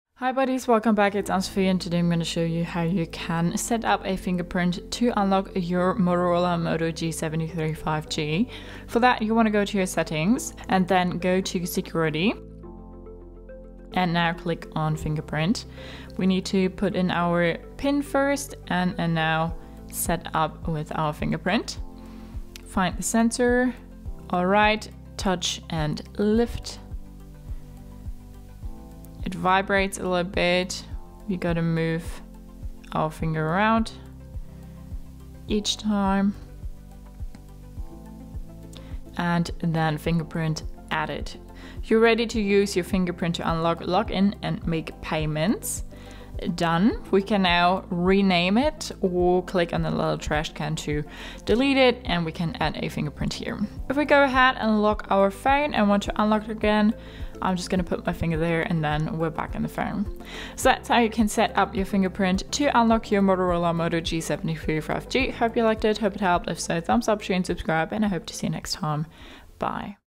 Motorola moto g73 5G - How to set up Fingerprint • 📱 • 👆🏼 • 🔐 • Tutorial
Do you want to set up a fingerprint ID on your Motorola moto g73 5G, so you can unlock it? Together, we will take a look on how that works. The Moto g73 5G recognizes your dactylogram via the sensor and unlocks itself for your use. All for your security.
Have fun setting up your fingerprint and enjoy the video!

Another tip: Be sure to "save" several fingers so that you always have an alternative for unlocking, for example, if you lose a finger or forget it at home... 😉